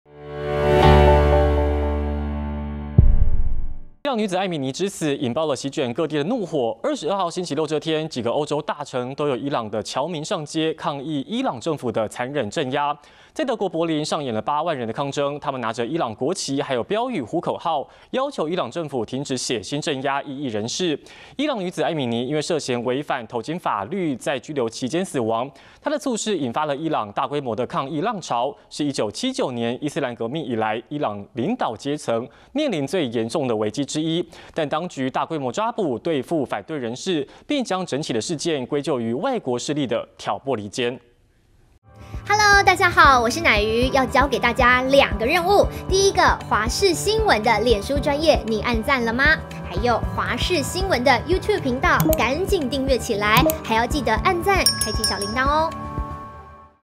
"艾米尼之死"掀怒火 各國伊朗僑民上街抗議｜華視新聞 20221023
伊朗女子艾米尼之死，引爆席捲各地的怒火，22日星期六這天，幾個歐洲大城都有伊朗僑民上街，抗議伊朗政府的殘忍鎮壓。